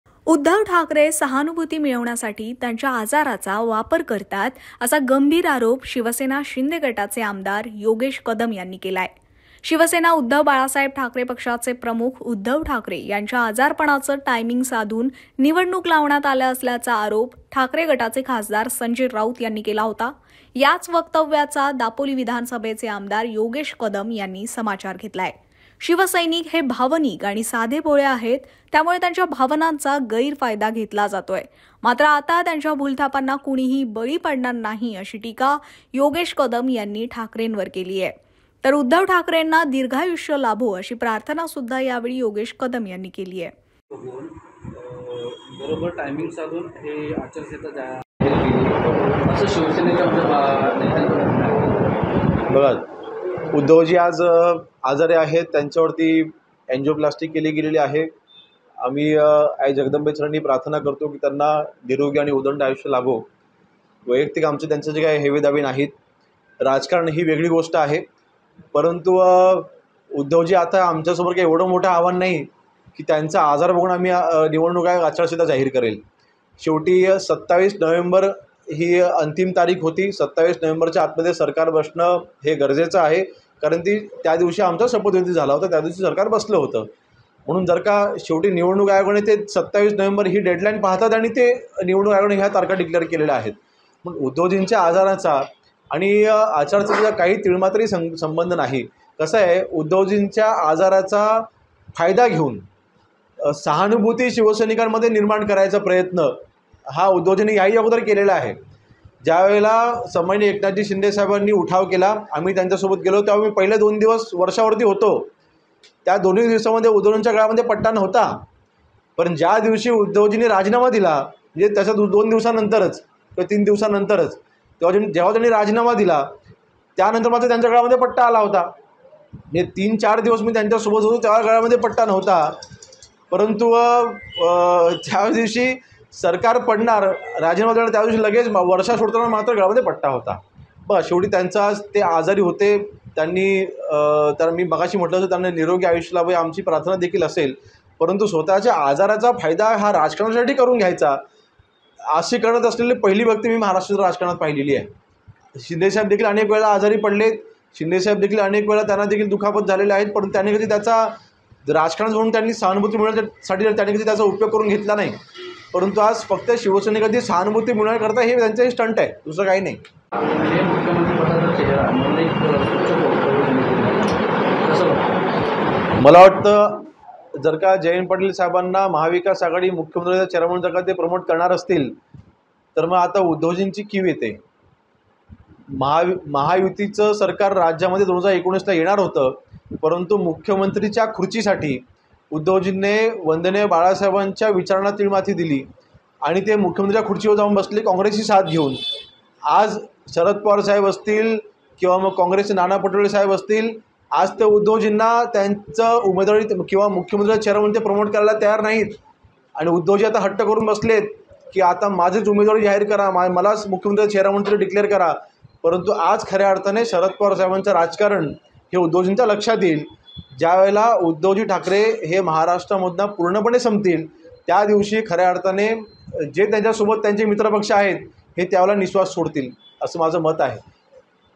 Yogesh Kadam | सहानुभूतीसाठी ठाकरेंचा स्टंट, पवारांपासून सावध व्हा; आजारपणावर योगेश कदमांची टीका
उद्धव ठाकरे सहानुभूती मिळवण्यासाठी त्यांच्या आजाराचा वापर करतात, असा गंभीर आरोप शिवसेना शिंदे गटाचे आमदार योगेश कदम यांनी केलाय. उद्धव ठाकरे यांच्या आजारपणाचे टायमिंग साधून निवडणूक लावण्यात आले असल्याचा आरोप ठाकरे गटाचे खासदार संजय राऊत यांनी केला होता, याच वक्तव्याचा  दापोली विधानसभेचे आमदार योगेश कदम यांनी समाचार घेतलाय. शिवसैनिक हे भावनिक आणि साधे भोळे आहेत. त्यामुळे त्यांच्या भावनांचा गैरफायदा घेतला जात आहे. मात्र आता त्यांच्या भूलथापांना कोणीही बळी पडणार नाही, अशी टीका योगेश कदम यांनी ठाकरेंवर केलीय. तर उद्धव ठाकरेंना दीर्घायुष्य लाभो अशी प्रार्थनाही यावेळी योगेश कदम यांनी केलीय.